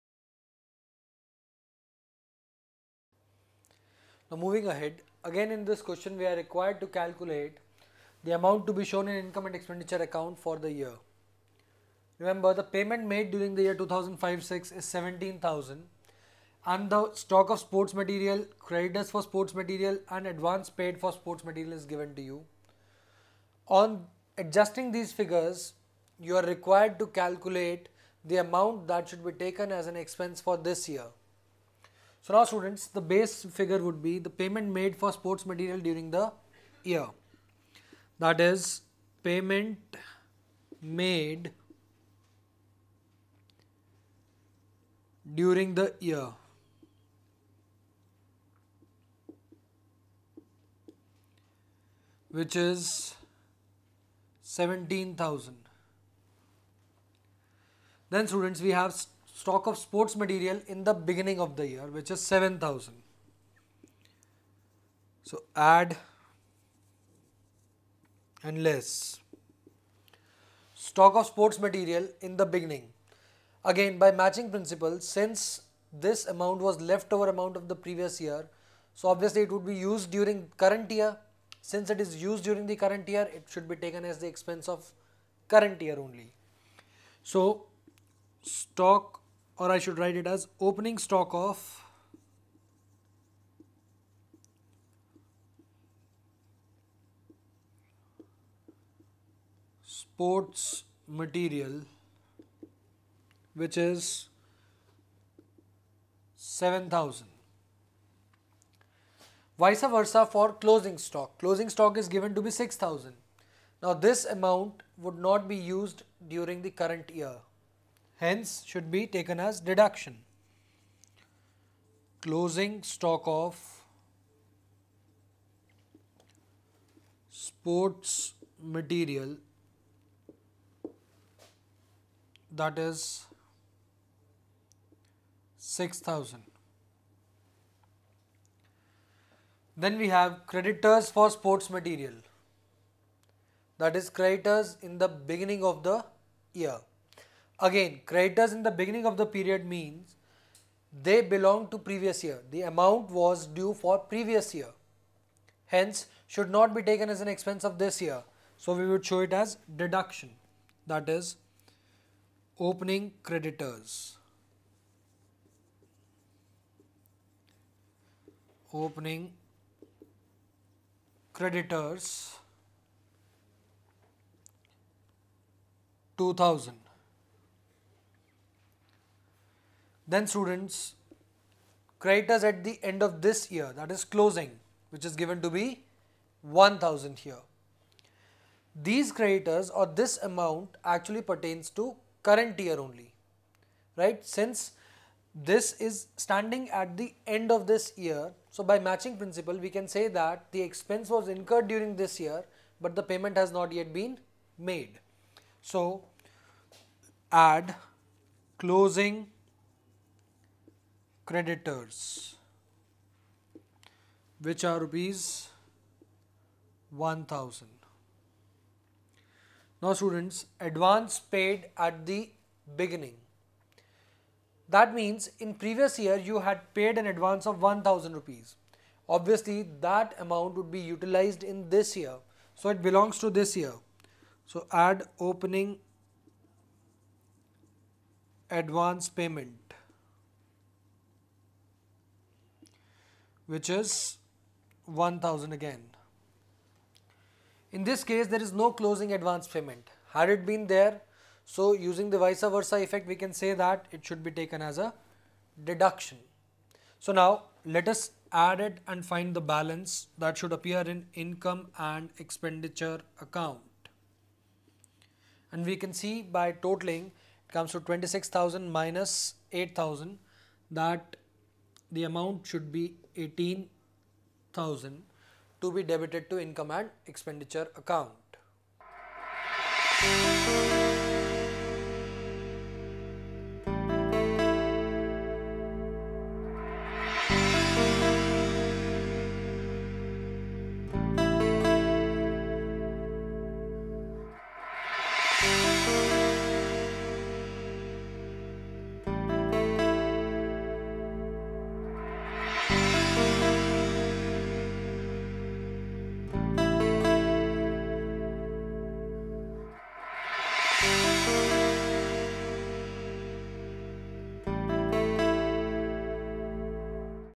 CBSE Class 12 Accounts Accounting for not for Profit Organisation Illustration-VI |Extraminds
CBSE Class 12 Accounts
Chapter: Accounting for not for Profit Organisation
Topic: Illustration-VI
Extraminds